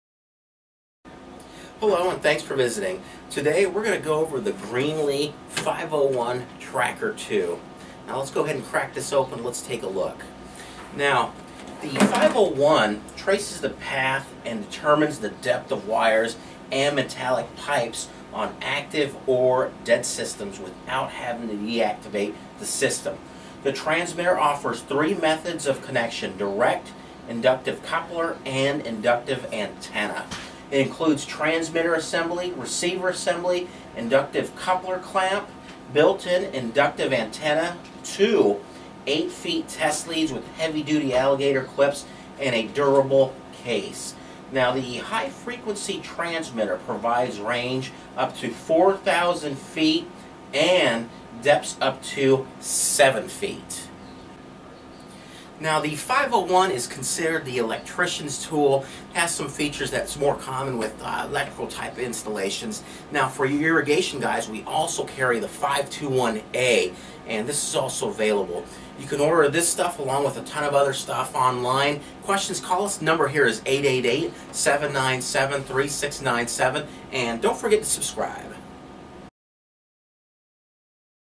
Greenlee 501 Tracker II Cable Locator
Traces the path and determines the depth of wires and metallic pipes on active or dead systems without having to deactivate the system. Lightweight receiver provides audible and visual indications of signal strength. Transmitter output and receiver sensitivity are fully adjustable for maximum accuracy. Transmitter offers three methods of connection: direct, inductive coupler, and inductive antenna. Includes transmitter assembly, receiver assembly, inductive coupler (clamp), built-in inductive antenna, two 8 ft. (2.4 m) test leads with heavy-duty alligator clips and durable polyethylene case. High frequency transmitter provides range up to 4,000 ft. (1,219 m) and depths up to 7 ft. (2 m).
Receiver is 32 in. long (813 mm). Battery operated (eight AA and one 9V battery are included) Molded carrying case included. 1 year limited warranty.